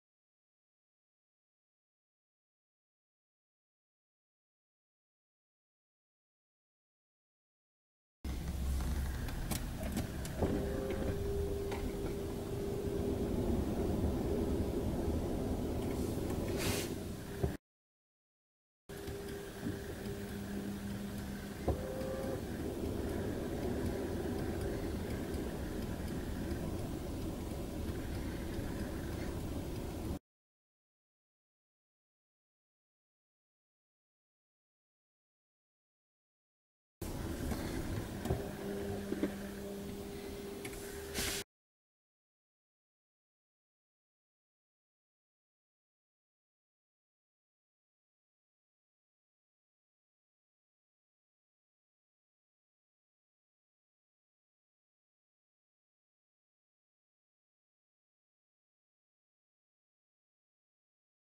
Mercedes W211 How to use hidden air condition (AC) menu
Gefilmt mit (Affilatelinks)!
GoPro Hero 8 Black
EDUTIGE Mikrofon
ZOOM H2N Audio-Rekorder
Racelogic Performance Box

Do it yourself: How to enter the hidden AC menu and a list of values displayed at the end of the video.

Caution: Be careful not to change any value!